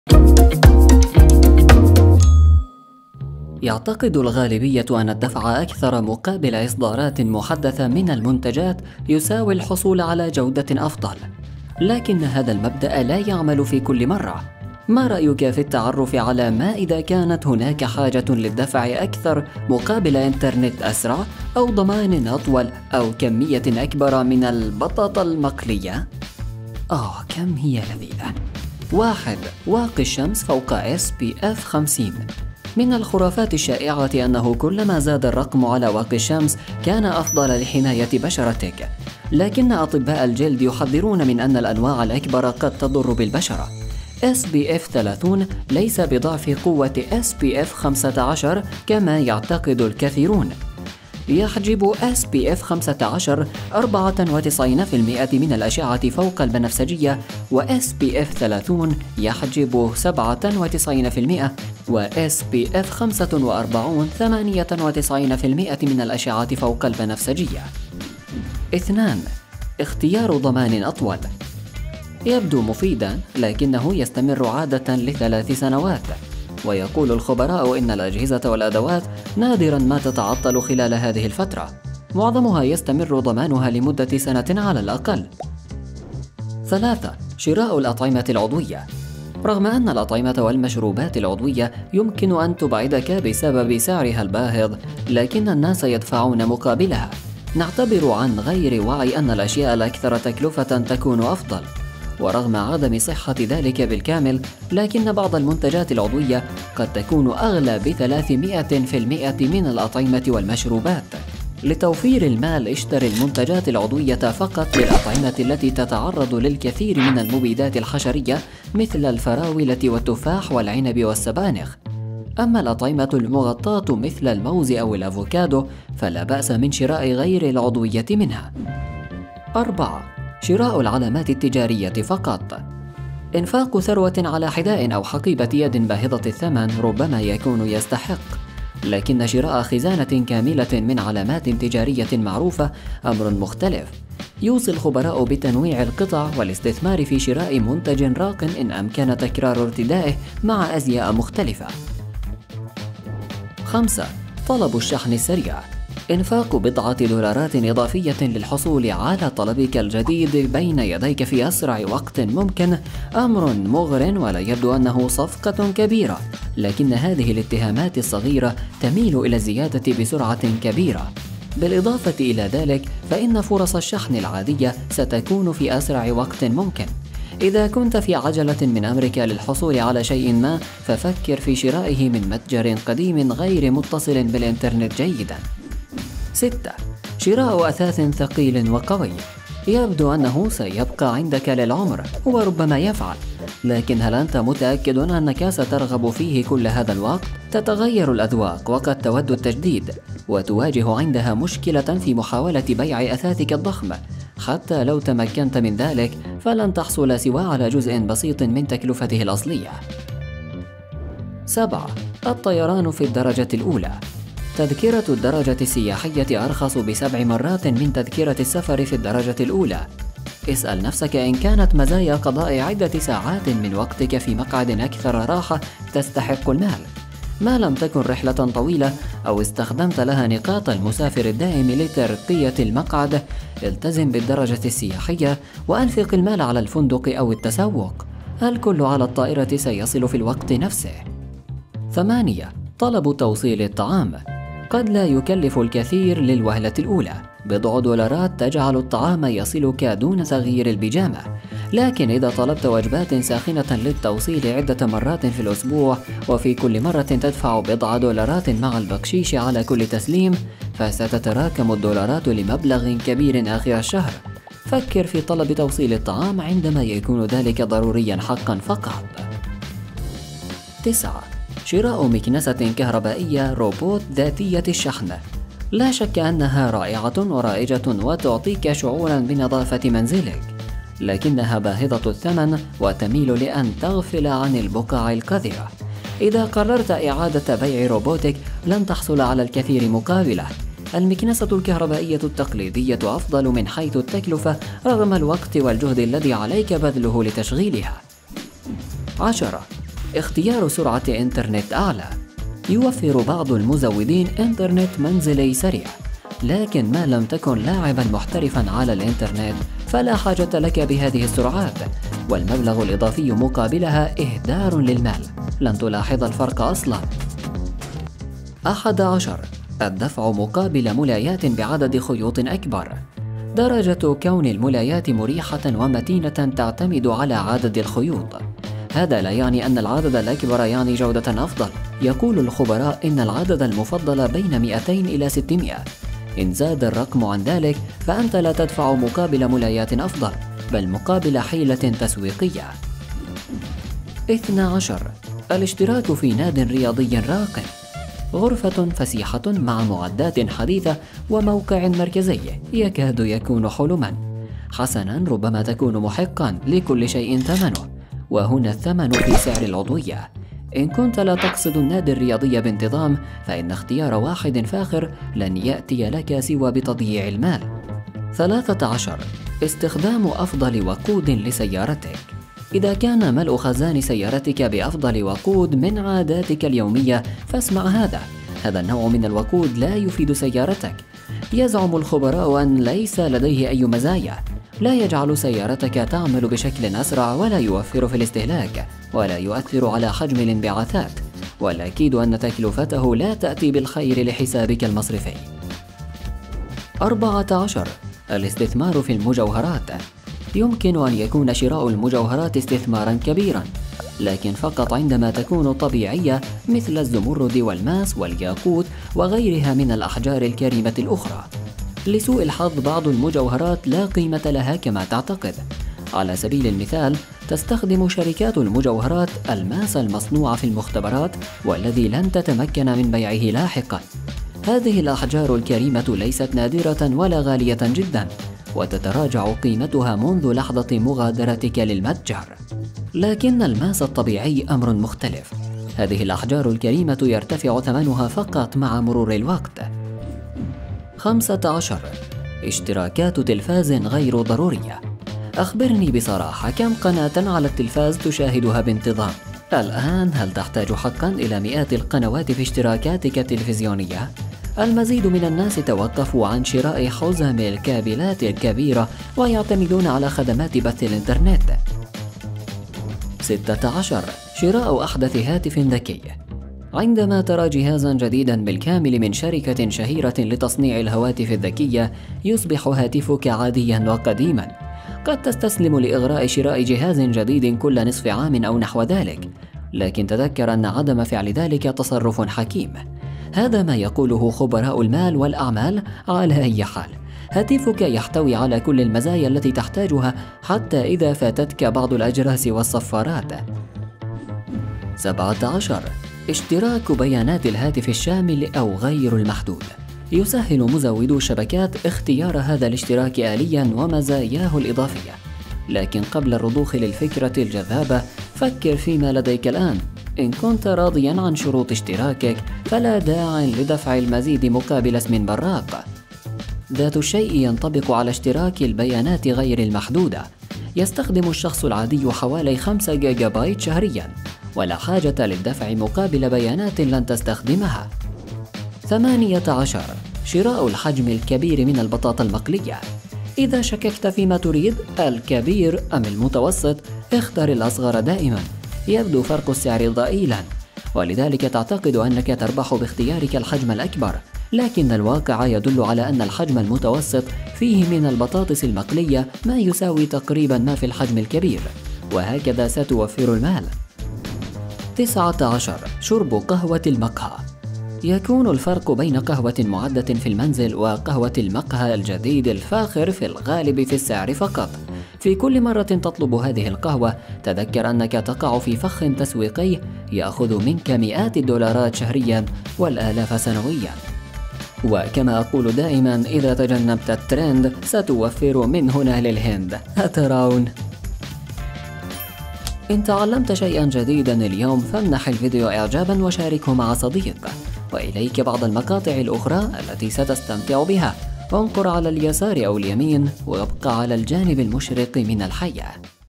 19 مرة عليك ألا تدفع فيها مقابل التحديث الأغلى
يعتقد الغالبية أن الدفع أكثر مقابل إصدارات "محدثة" من المنتجات يساوي الحصول على جودة أفضل. لكن هذا المبدأ لا يعمل في كل مرة. الأطعمة العضوية، على سبيل المثال. يمكن لبعضها أن تكون أغلى ب300٪ من الأطعمة والمشروبات. لكن هل تعرف أن الأطعمة المغطاة مثل الموز أو الأفوكادو، لا بأس من شرائها غير عضوية؟

مثال آخر: اختيار انترنت أسرع. يوفر بعض المزودين إنترنت منزلي سريع. لكن ما لم تكن لاعبً محترفا على الإنترنت، فلا حاجة لك بهذه السرعات، والمبلغ الإضافي مقابلها إهدار للمال. ما رأيك في التعرف على ما إذا كانت هناك حاجة للدفع أكثر مقابل إنترنت أسرع أو ضمان أطول أو كمية أكبر من البطاطا المقلية؟

الطوابع الزمنية:

واقي الشمس فوق  SPF 50 ـ 00:28

اختيار ضمان أطول 01:08

شراء الأطعمة العضوية 01:27

شراء العلامات التجارية فقط 02:09

طلب الشحن السريع 02:34

شراء أثاث قوي وثقيل 03:06

الطيران في الدرجة الأولى 03:31

طلب توصيل الطعام 04:01

شراء مكنسة كهربائية روبوت 04:30

سرعة إنترنت أعلى 04:59

الدفع مقابل ملايات بعدد خيوط أكبر 05:19

الاشتراك في نادي رياضية راقي 05:44

أفضل وقود لسيارتك ز 06:09

الاستثمار في المجوهرات 06:36

اشتراكات تلفاز غير ضرورية 07:20

شراء أحدث هاتف ذكي 07:43

اشتراك بيانات الهاتف الشامل أو غير المحدود 08:14

شراء الحجم الكبير من البطاط المقلية 08:49

شرب قهوة المقهى 09:14